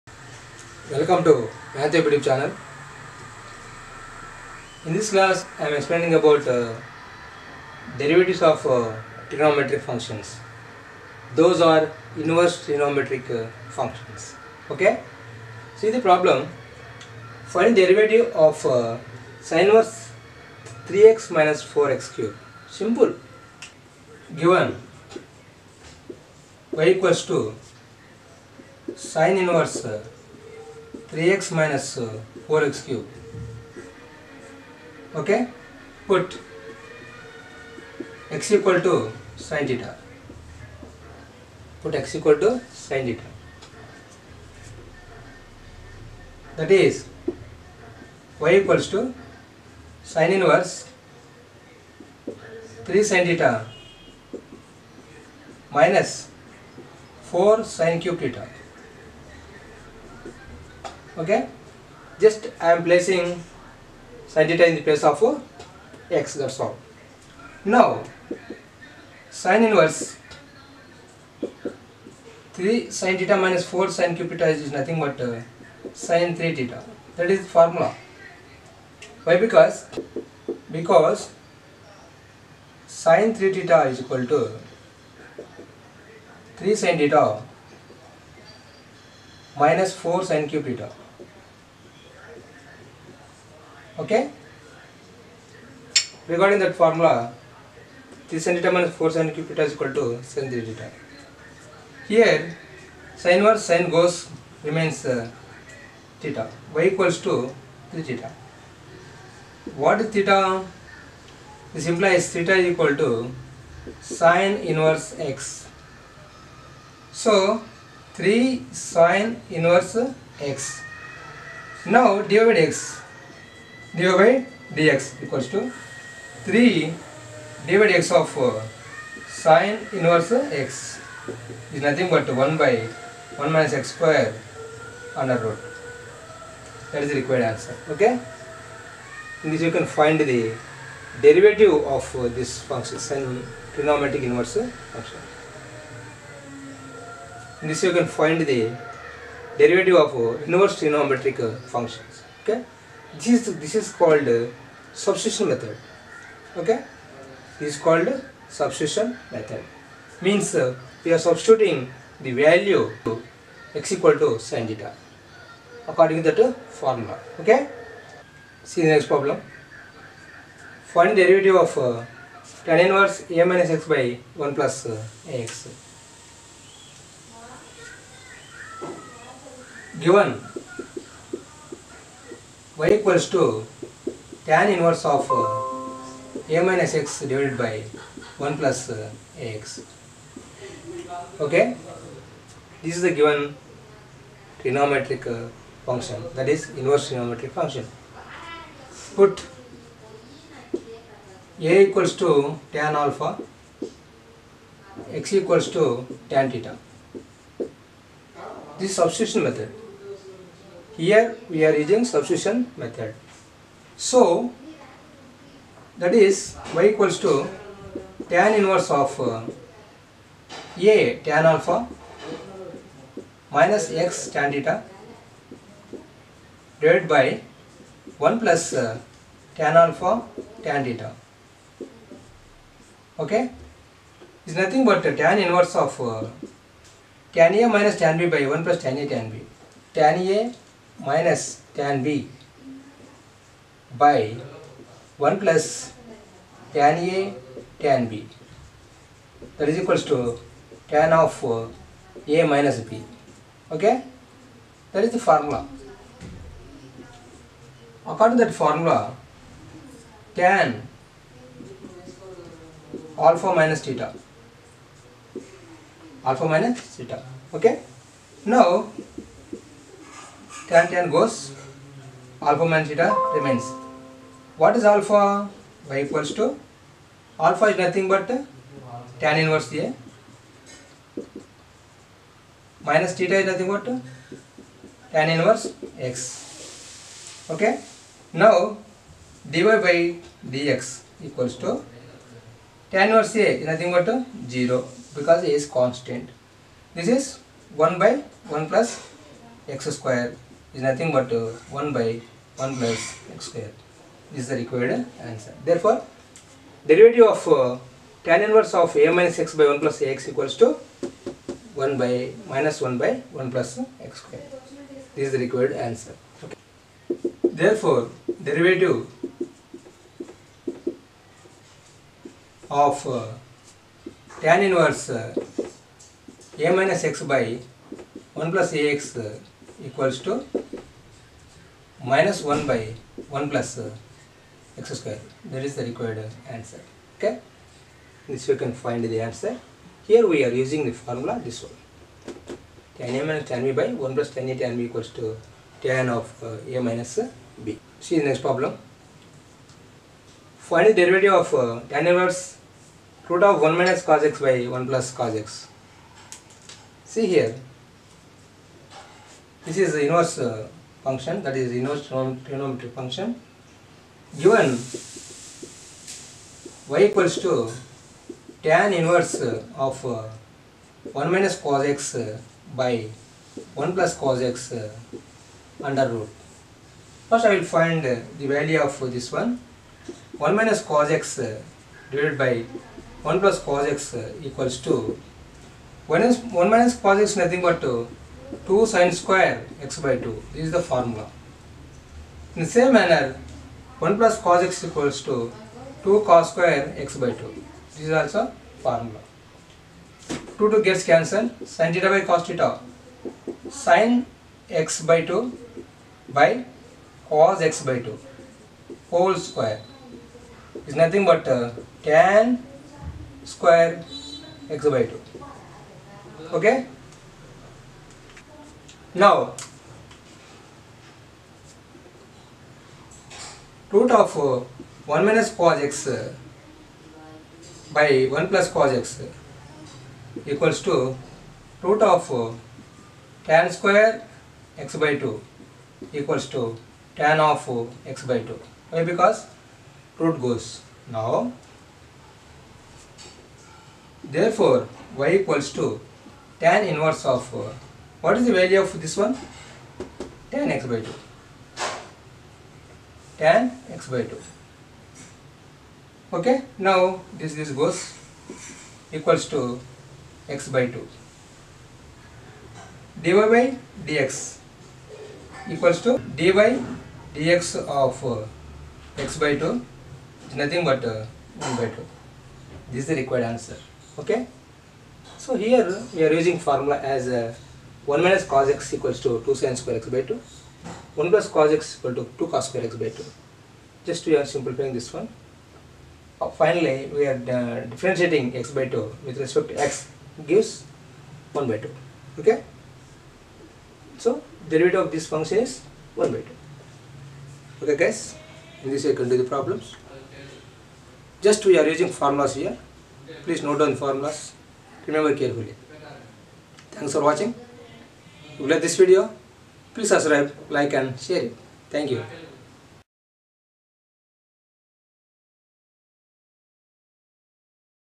Differential Equations part 11, Derivatives, Inter math 1B, K.Narayana reddy, JEE, AIEEE, NEET, B E.
Here am explaining about the Derivative of the inverse trigonometric functions.